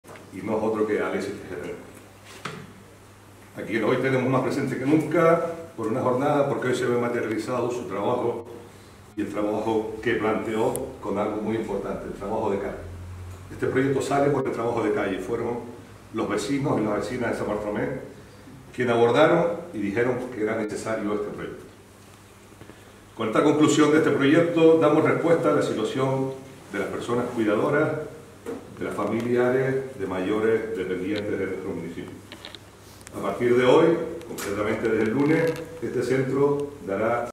El pionero servicio del Centro de Respiro Familiar de San Bartolomé es ya una realidad
Isidro Pérez subraya la novedad en Canarias de este centro de día puesto en marcha a través del concierto social. El alcalde también ha tenido palabras emocionadas para Alexis Tejera.